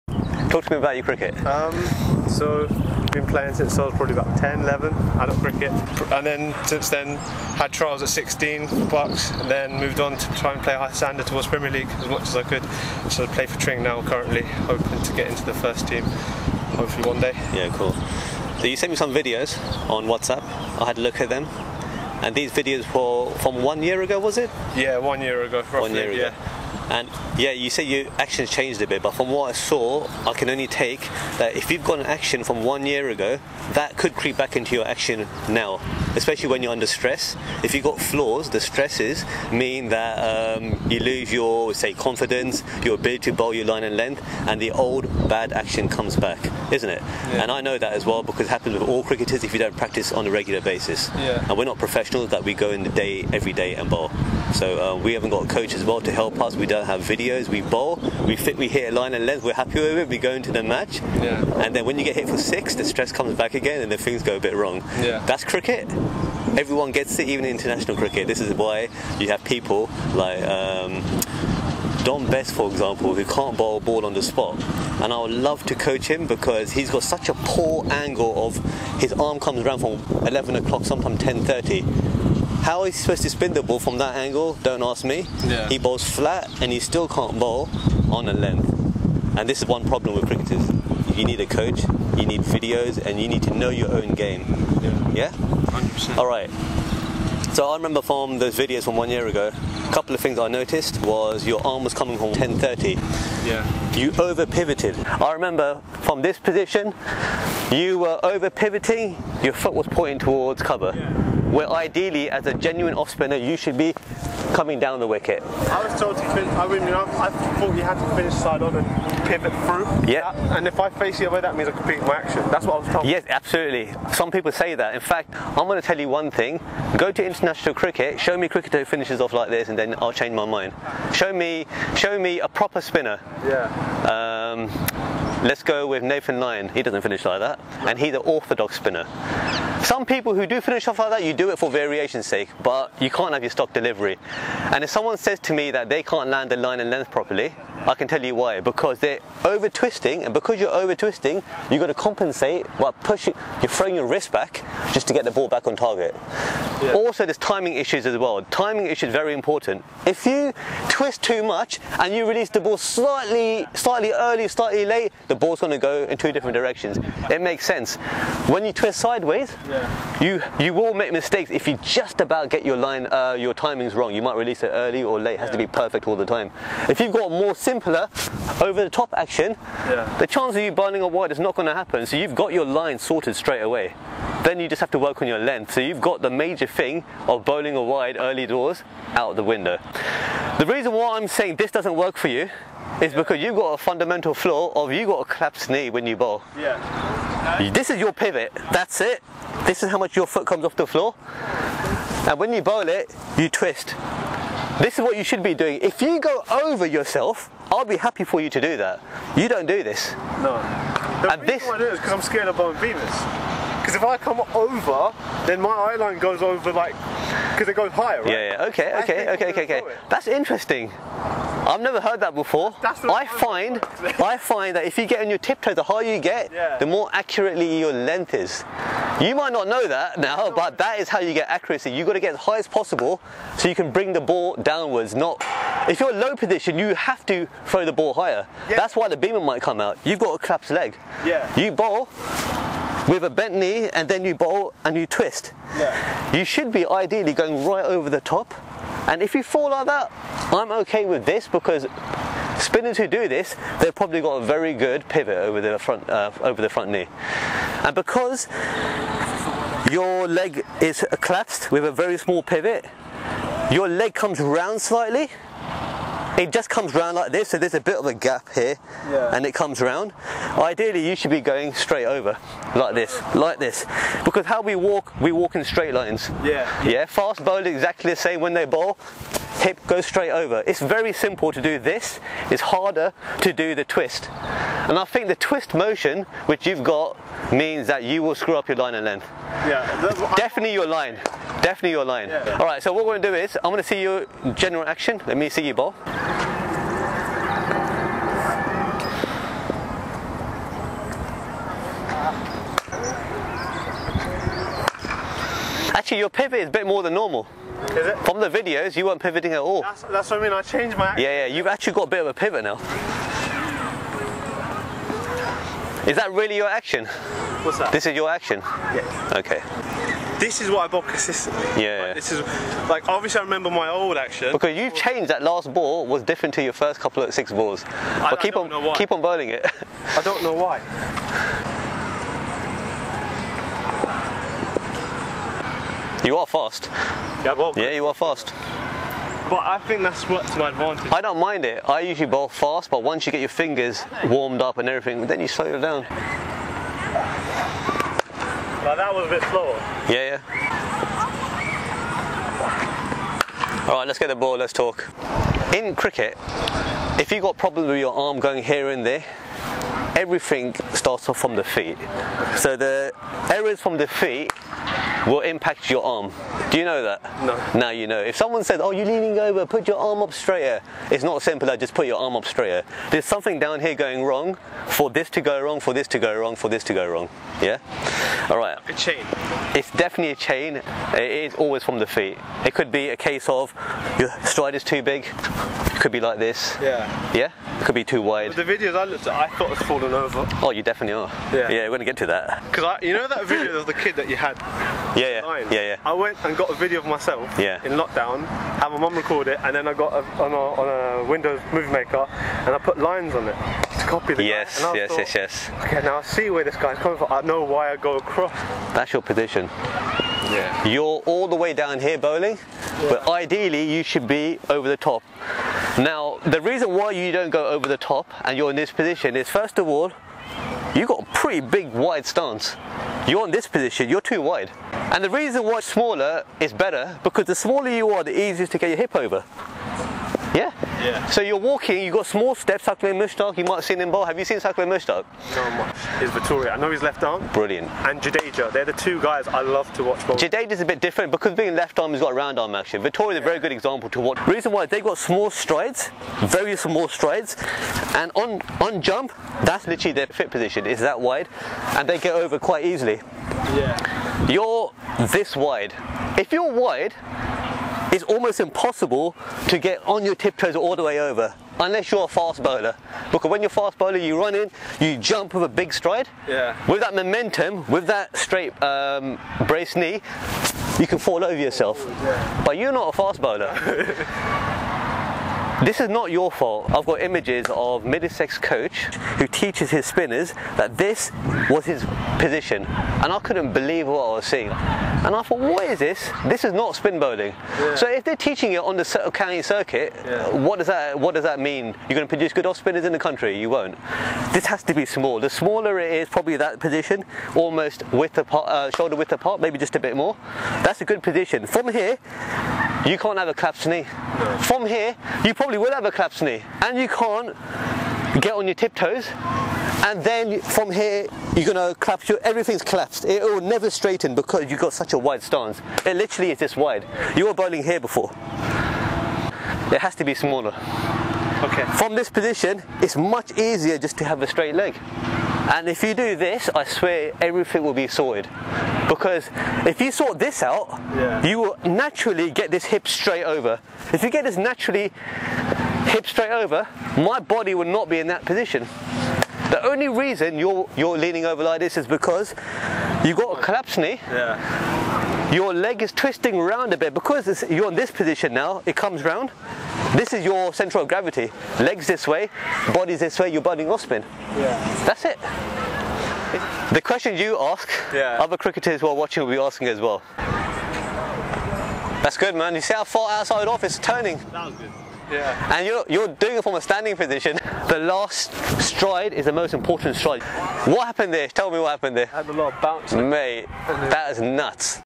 P1 Off Spin Flaws 🔴 Live masterclass cricket coaching session
Here is another live off spin coaching session but this time it's broken down into 3 parts.